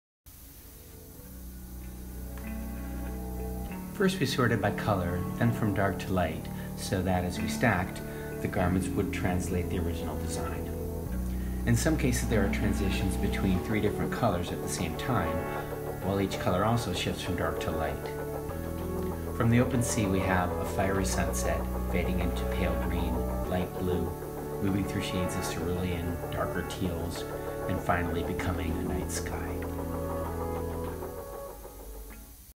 Fabrication for 'You are my other me,' 2019 by Derick Melander (Part 2)
This piece was commissioned by Swire for Starstreet Precinct and on view at 3 Pacific Place, Wan Chzi, Hong Kong, till April 28th. The title, 'You are my other me,' is an ancient Mayan proverb. Wonderful video and production seen here by Shawn Griffin & Zach Sung (IG: @zachary_sung).